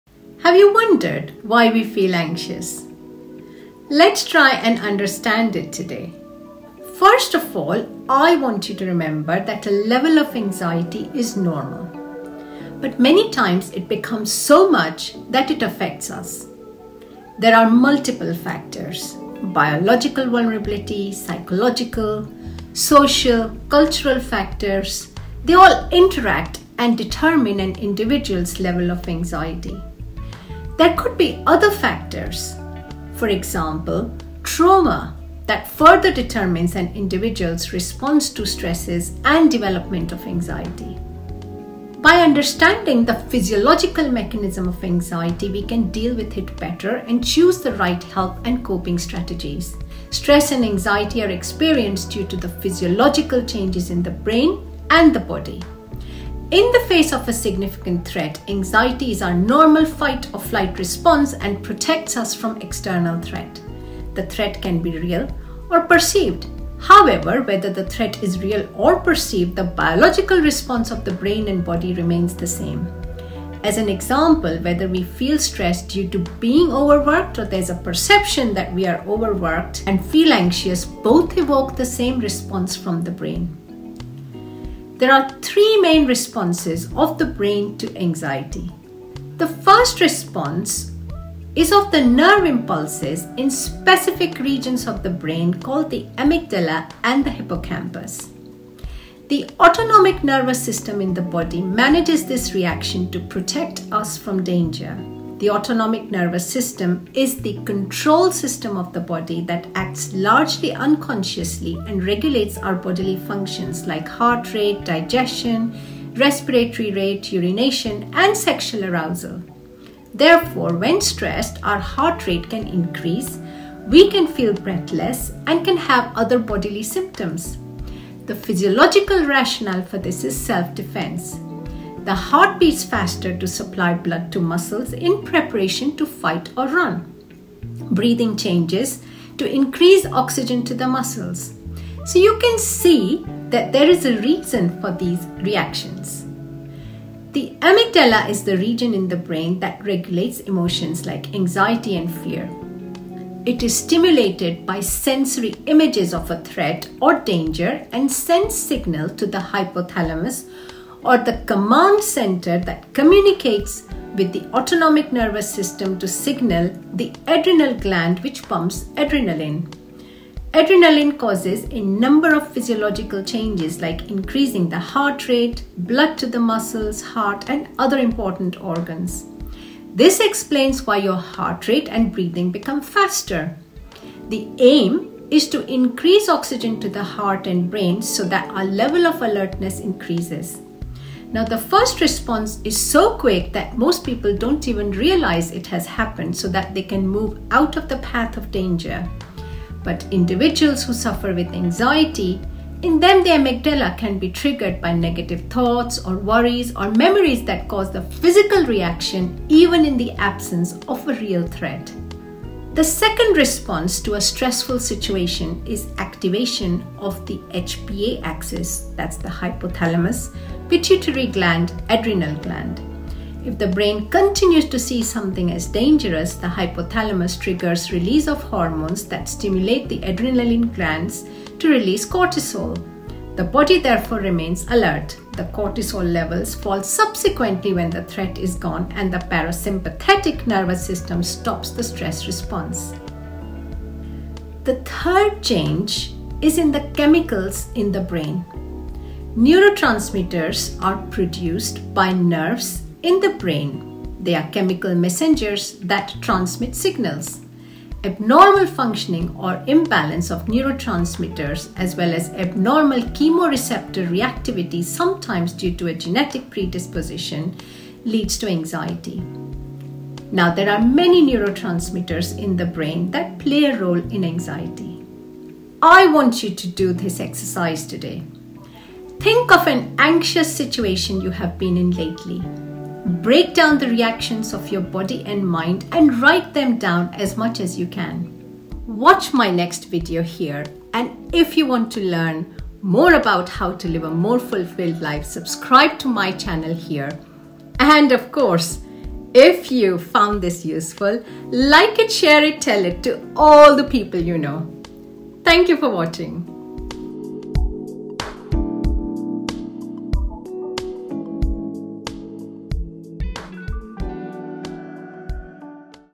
How The Body Copes With Anxiety and How To Control It
Do you have negative thoughts, worries and memories that spike adrenaline into your blood and you feel your heart rate and breathing become faster? On occasion, you experience these reactions even in the absence of the real threat.

This short video will explain how the brain and body reacts when you are anxious or stressed.
Why is this important? Because next time you experience the same body reactions, you will be able to calm down quickly and respond accordingly to the situation knowing that these are your body's protective mechanisms.
Today I want you to think of an anxious situation you’ve been in lately. Break down the reactions of your body and mind, as you’ve learned them from the video. Write them down.